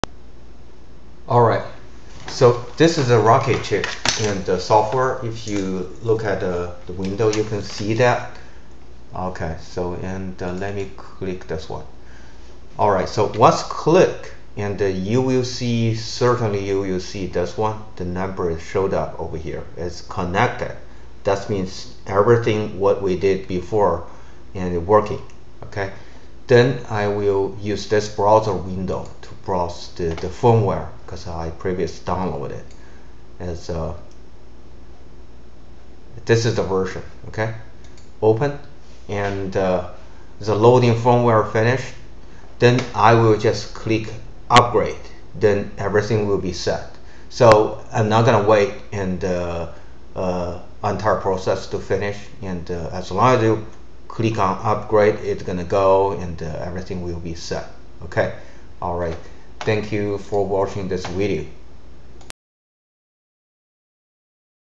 Upgrade Tronsmart MK908 II Firmware - Rockchip
Rockchip_Batch_Tool_v1.6
Use this tool to flash the firmware to MK908